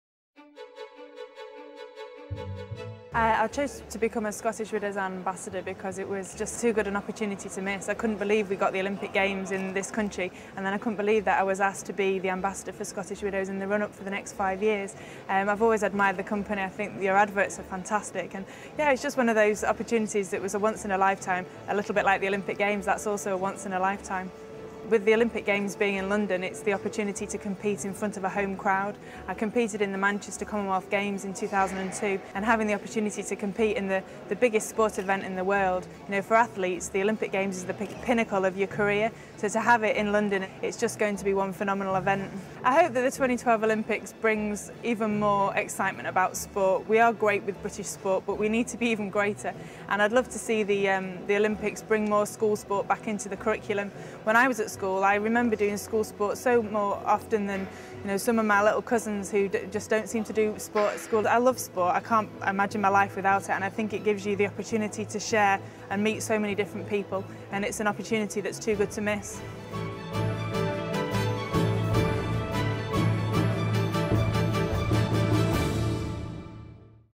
Scottish Widows - Olympic Ambassadors Part 2 - Sarah Storey
Our promotional video for Scottish Widows Olympic Ambassadors.

The brief was to create a video Showcasing the Scottish Widows Olympic Ambassadors. This video features x7 gold medalist and competing champion Sarah Storey talking about the 2012 London Olympic Games.

Our involvement in this project range from film production, filming, lighting, editing etc.